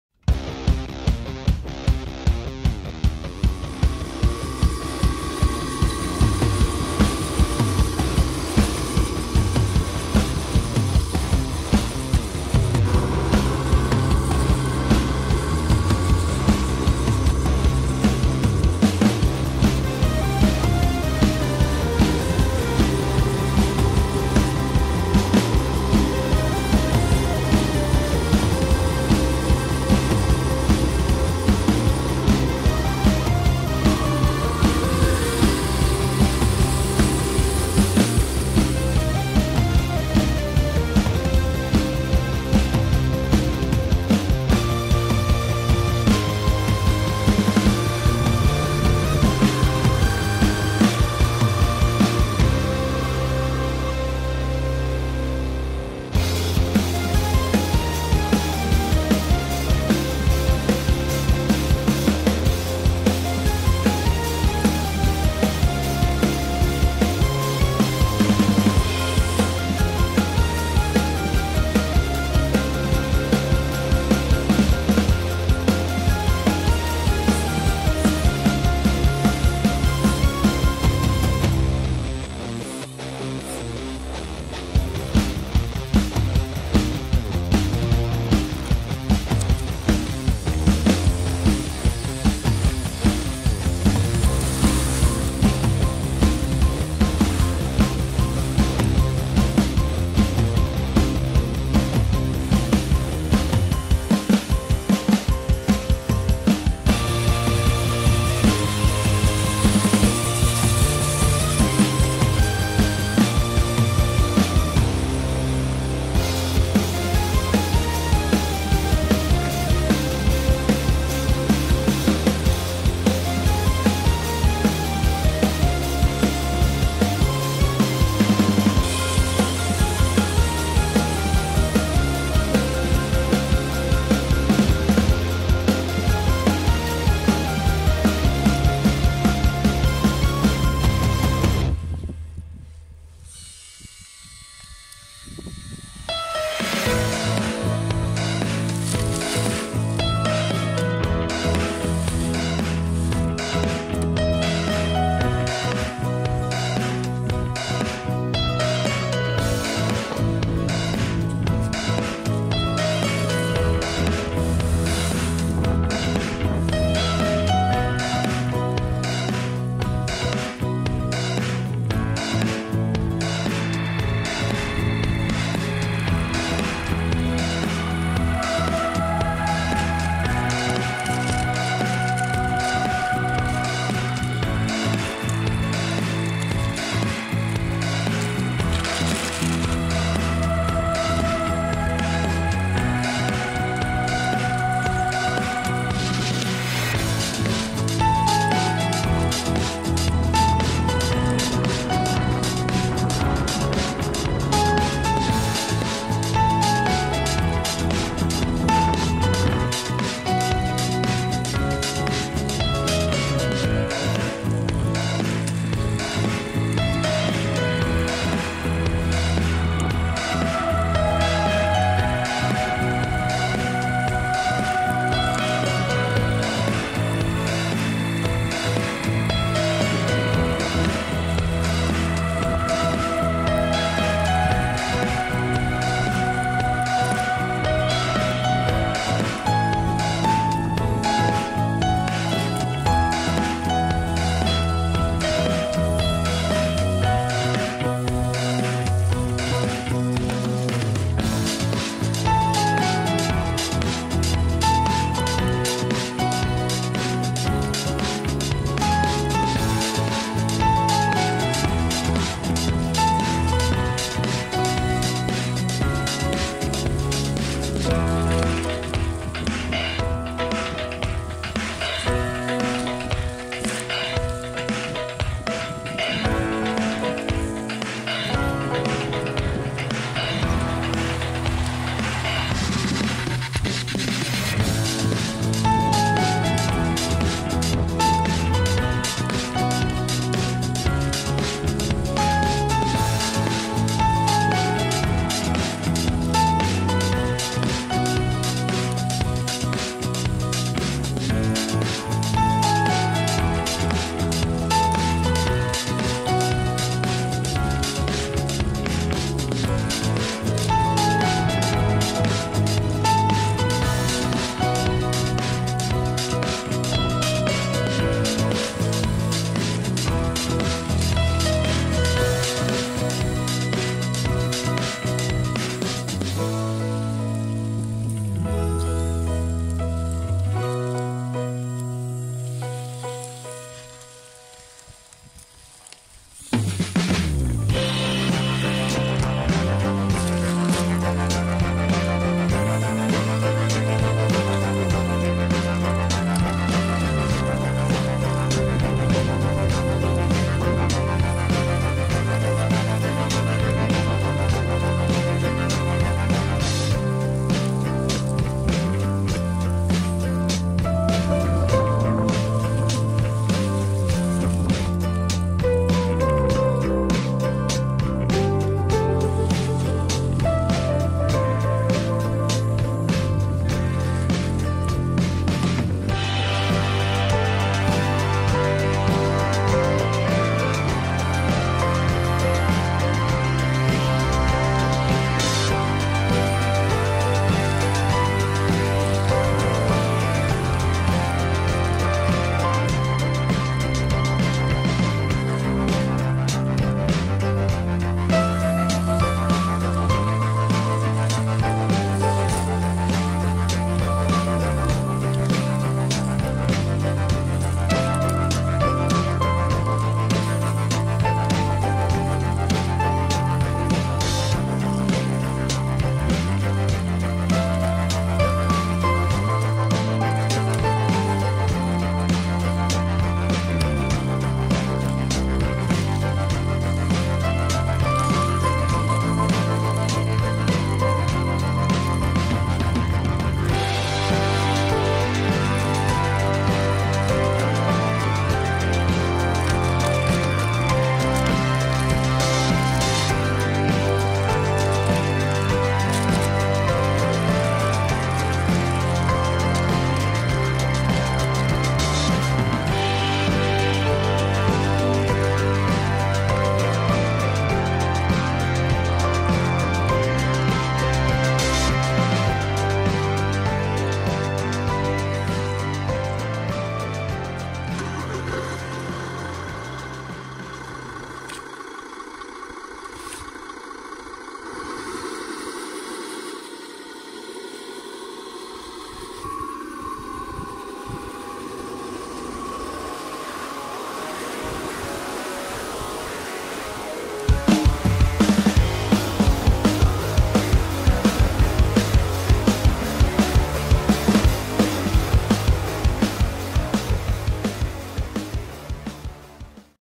RC Construction Equipment - 3D PRINTED Bulldozer and two Mercedes Trucks
Showcase video of my 3d printed Bulldozer